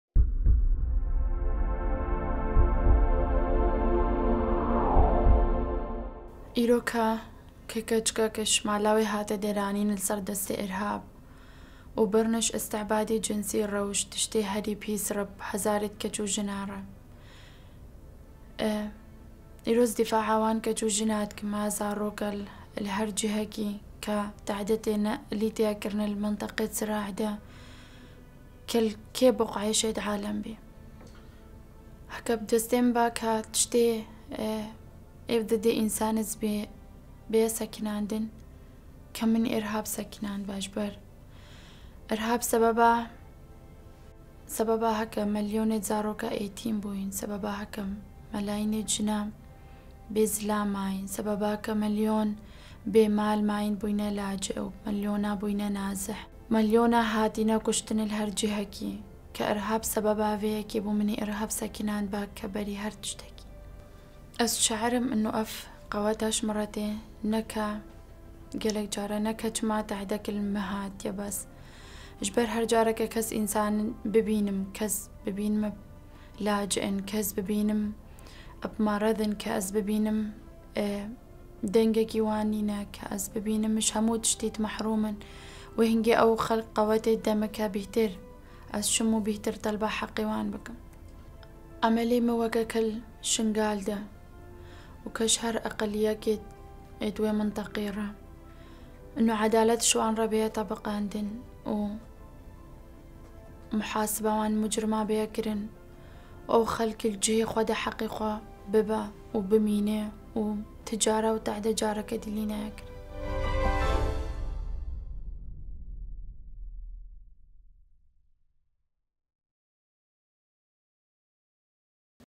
Nadia Murad - Human Rights Activist
Nadia Murad is a Yazidi human rights activist from Sinjar in northern Iraq. Join us and "Stand up for someone's rights today."